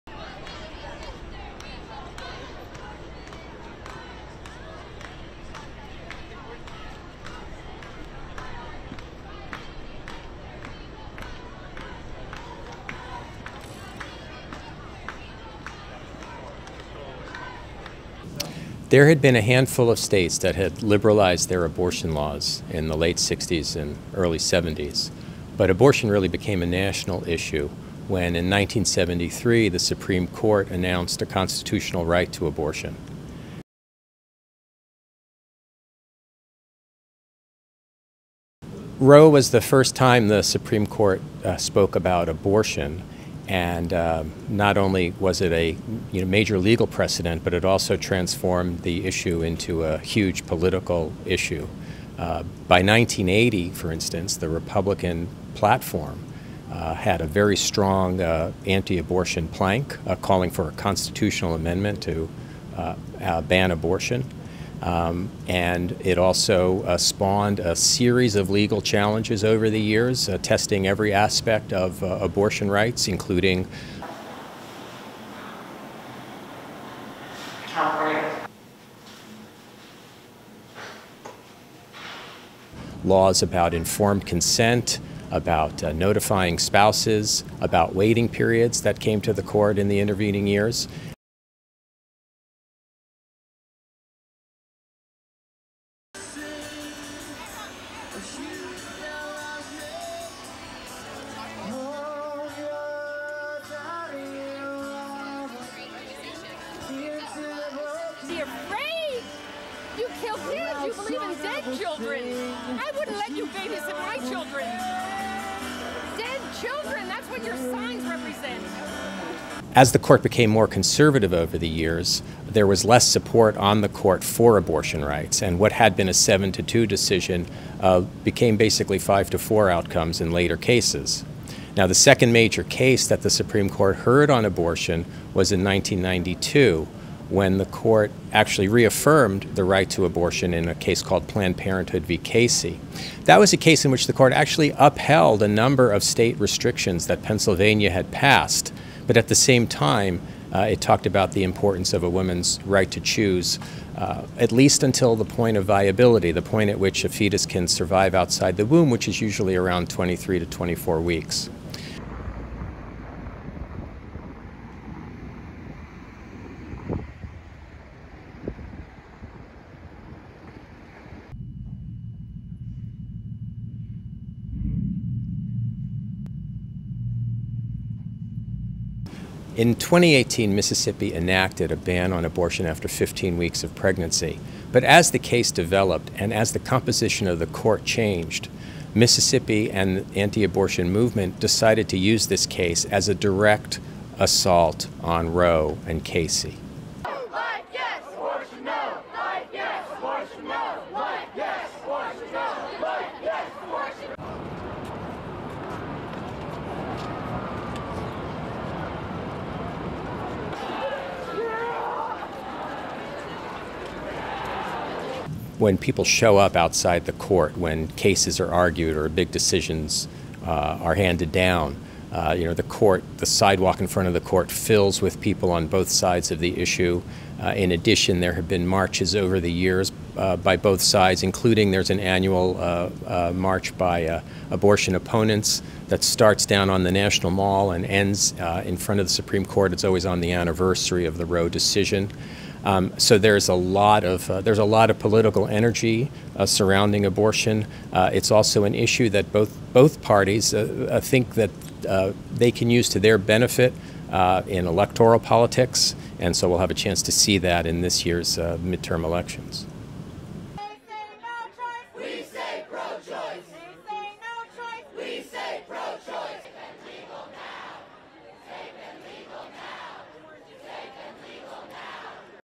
Political history of Supreme Court abortion cases
RESTRICTION SUMMARY:
ASSOCIATED PRESS
Washington, DC - 13 June 2022
1. Wide of the people, fencing in front of the U.S. Supreme Court
ASSOCIATED PRESS
Washington, DC - 14 June 2022
2. SOUNDBITE (English) Mark Sherman, Associated Press, Supreme Court Reporter:
"There had been a handful of states that had liberalized their abortion laws in the late sixties and early seventies. But abortion really became a national issue when in 1973, the Supreme Court announced a constitutional right to abortion."
ASSOCIATED PRESS
ARCHIVE: Washington, DC - 26 April 1989
3. STILL of Norma McCorvey, Jane Roe in the 1973 court case, left, and her attorney Gloria Allred hold hands as they leave the Supreme Court building
ASSOCIATED PRESS
4. SOUNDBITE (English) Mark Sherman, Associated Press, Supreme Court Reporter:
"Roe was the first time the Supreme Court spoke about abortion. And not only was it a major legal precedent, but it also transformed the issue into a huge political issue. By 1980, for instance, the Republican platform had a very strong anti-abortion plank calling for a constitutional amendment to ban abortion. And it also spawned a series of legal challenges over the years, testing every aspect of abortion rights, including..."
ASSOCIATED PRESS
ARCHIVE: Louisville, Kentucky – 15 April 2022
5. Various inside Planned Parenthood
ASSOCIATED PRESS
6. SOUNDBITE (English) Mark Sherman, Associated Press, Supreme Court Reporter:
"Laws about informed consent, about notifying spouses, about waiting periods that came to the court in the intervening years."
ASSOCIATED PRESS
ARCHIVE: Washington DC - 3 May 2022
7. STILL of a demonstrator holding up a sign with pictures of Supreme Court Justices Clarence Thomas, Brett Kavanaugh, Samuel Alito, Amy Coney Barrett, and Neil Gorsuch, during a protest outside of the U.S. Supreme Court
ASSOCIATED PRESS
ARCHIVE: Washington, DC - 30 November 2005
8. Various of pro-abortion and anti-abortion rights protesters in front of Supreme Court
ASSOCIATED PRESS
9. SOUNDBITE (English) Mark Sherman, Associated Press, Supreme Court Reporter:
"As the court became more conservative over the years, there was less support on the court for abortion rights. And what had been a 7 to 2 decision became basically 5 to 4 outcomes in later cases. Now, the second major case that the Supreme Court heard on abortion was in 1992, when the court actually reaffirmed the right to abortion in a case called Planned Parenthood v Casey. That was a case in which the court actually upheld a number of state restrictions that Pennsylvania had passed. But at the same time, it talked about the importance of a woman's right to choose, at least until the point of viability, the point at which a fetus can survive outside the womb, which is usually around 23 to 24 weeks."
ASSOCIATED PRESS
ARCHIVE: Jackson, Mississippi - 3 May 2022
10. Wide of sign outside an abortion clinic
11. Close of a flyer for women clinic experience survey
ASSOCIATED PRESS
12. SOUNDBITE (English) Mark Sherman, Associated Press, Supreme Court Reporter:
"In 2018, Mississippi enacted a ban on abortion after 15 weeks of pregnancy. But as the case developed and as the composition of the court changed, Mississippi, an anti-abortion movement decided to use this case as a direct assault on Roe and Casey."
ASSOCIATED PRESS
Washington, DC - 22 January 1997
13. Various of March for Life, anti-abortion demonstrators holding signs near Supreme Court
ASSOCIATED PRESS
ASSOCIATED PRESS